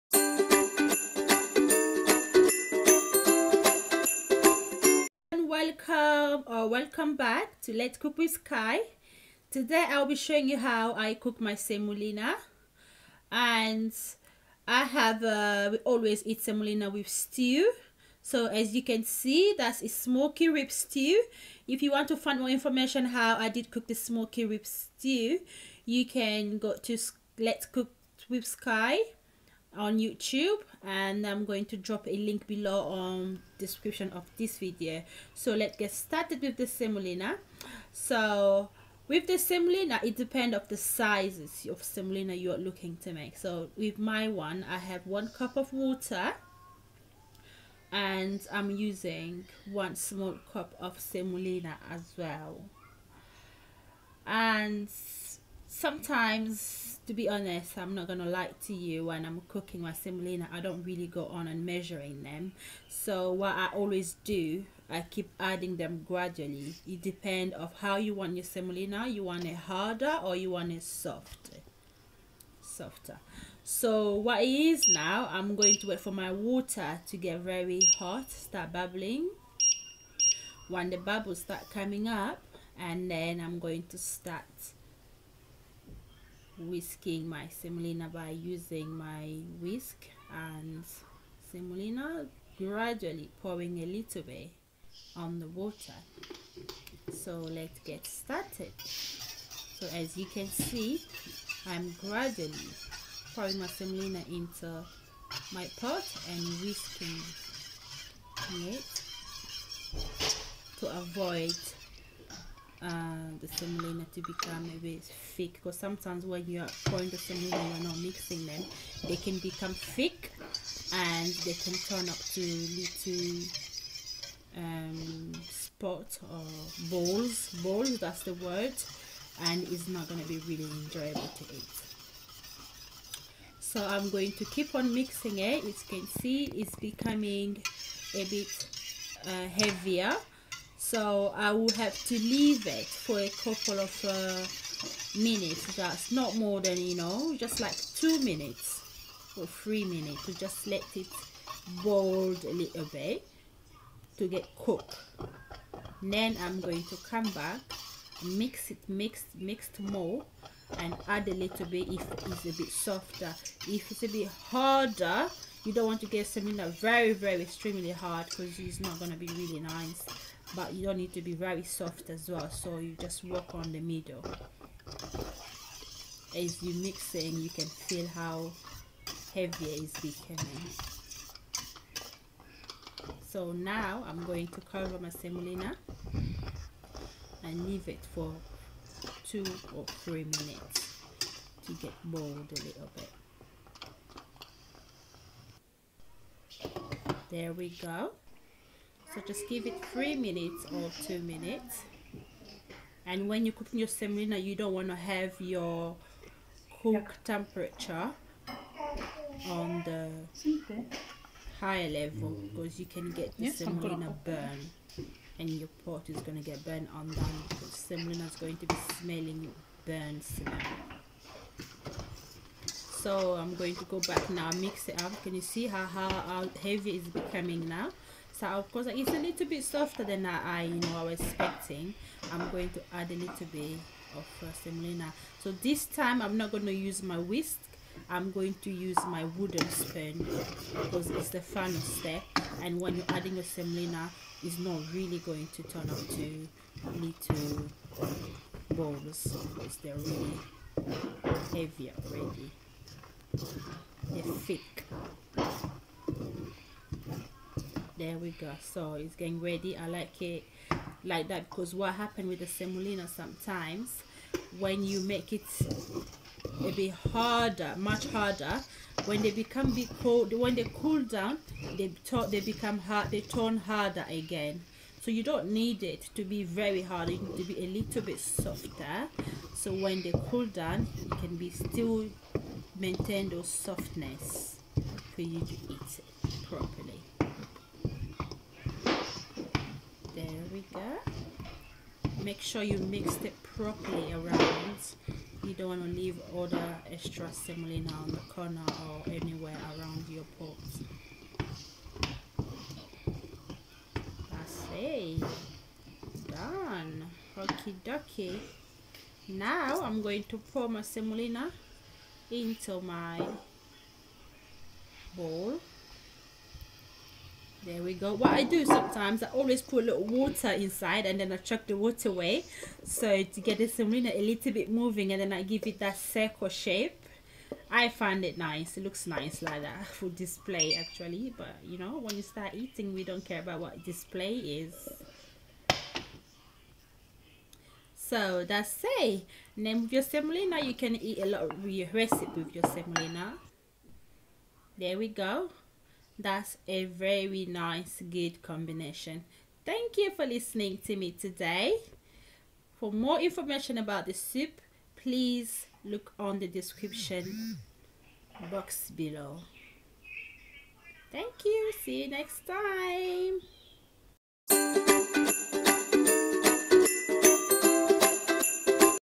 How to cook Semolina (fufu) without farina.
Ingredients:

1 medium cup of water.

1 small cup Semolina course.

Both the water and semolina quantities can vary depending on the portion you are looking to make.

Equipments:

Cooking pot.
Whisk.
Wooden spoon.

Or just visit my YouTube channel and search for easy way to cook smoky ribs stew.

Thankd for watching this video.